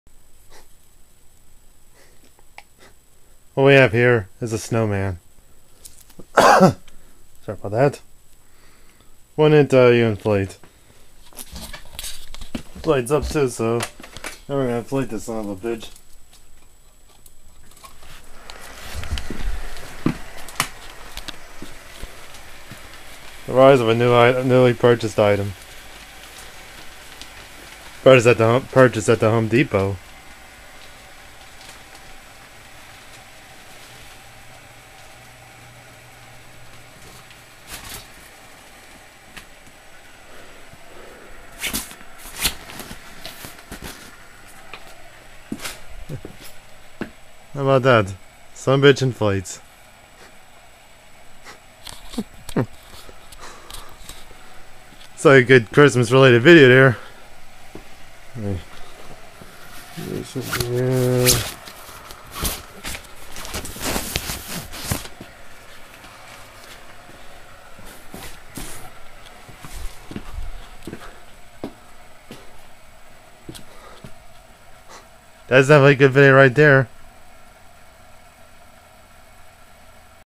Inflatable Snowman First Use
Just The First Use Of A Inflatable Snowman We Had Gotten From Home Depot The Day After Thanksgiving